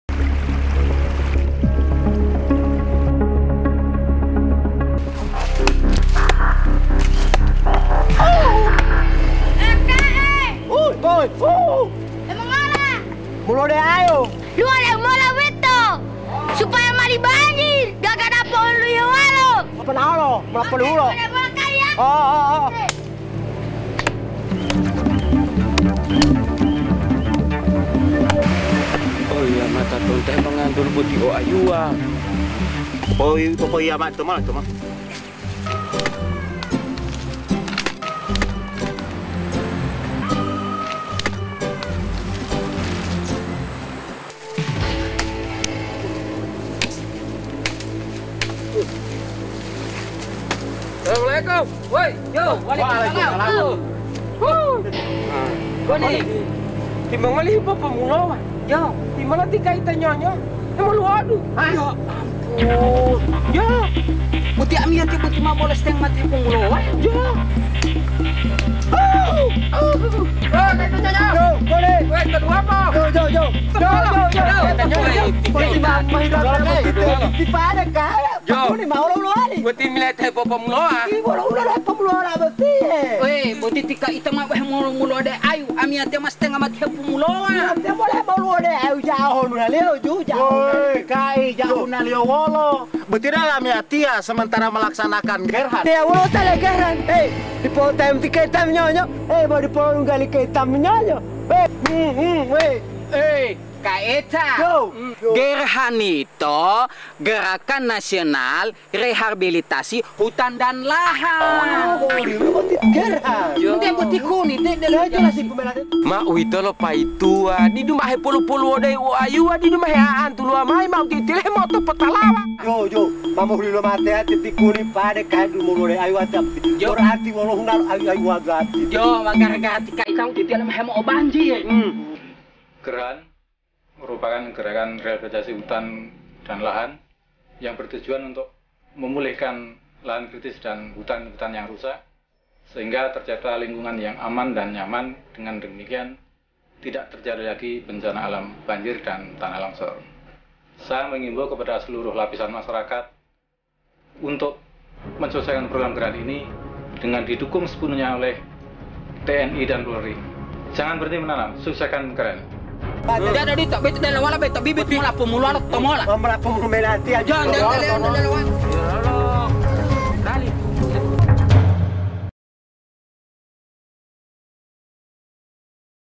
Siapa itu si Gerhan, dia belum tau Kaita Nyonyo???
Iklan Layanan Masyarakat : “Gerakan Nasional Rehabilitasi Hutan dan Lahan"
Persembahan : Balai Pengelola Daerah Aliran Sungai (BP-DAS) Provinsi Gorontalo